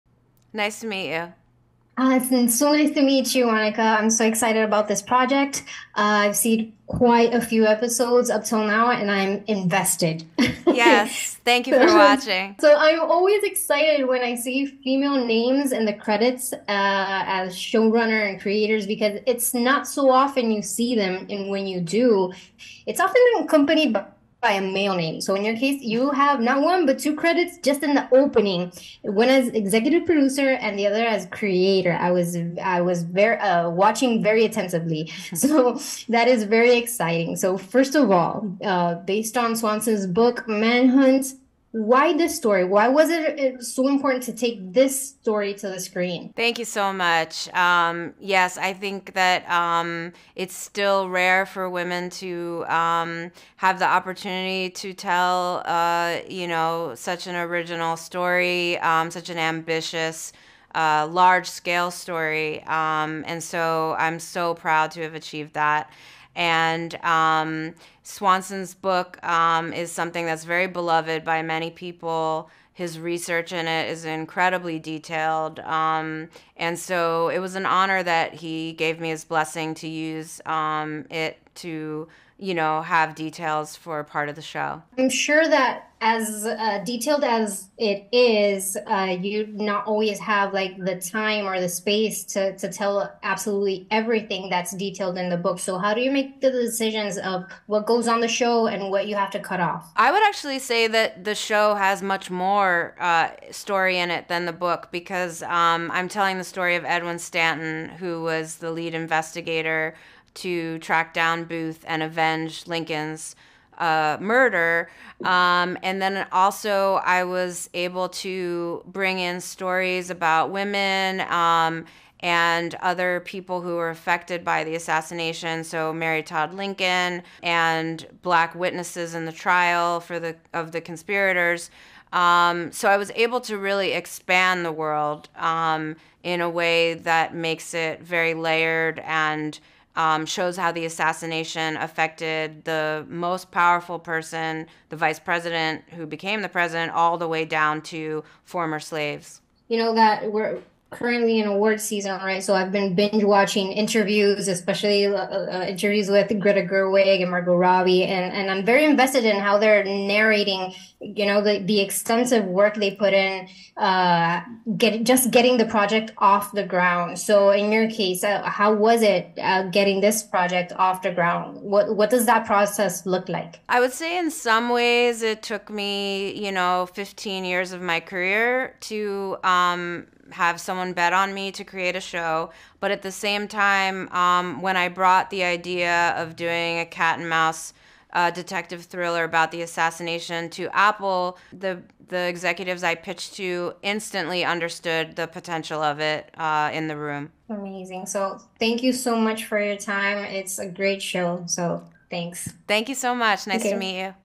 Monica Beletsky Interview for Manhunt, Apple TV Plus
Part historical fiction, part conspiracy thriller, Edwin Stanton delves into the aftermath of the first American presidential assassination-and the fight to preserve and protect the ideals that were the foundation of Lincoln's Reconstruction plans.